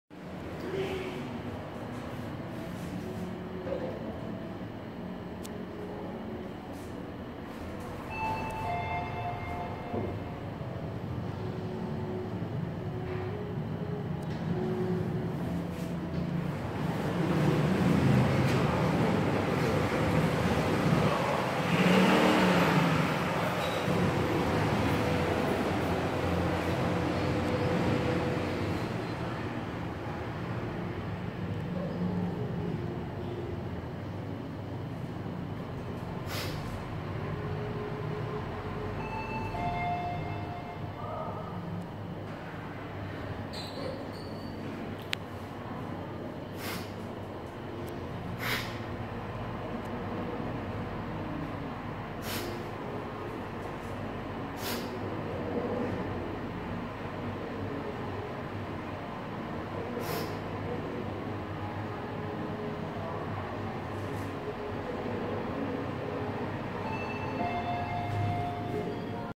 1993 Mitsubishi『Mod: 2017 ThyssenKrupp』 Service Elevator @ CentralPlaza Bangna, Bangkok
It's modded from generic Mitsubishi. Enjoy!
セントラルプラザバンナー店の業務用エレベーター
ลิฟท์ขนของทิสเซ่นครุปป์ที่เซ็นทรัลพลาซ่า บางนา จังหวัดกรุงเทพมหานคร

Manufacturer : ThyssenKrupp
Generation : Unknown
Capacity : XX
Loading : 2,500 kg
Floor Served : B2, B1, 1 - 6

Area - Bangna, Bangkok
Date Taken - February 17th, 2018
Oak TheElevator Hunter Channal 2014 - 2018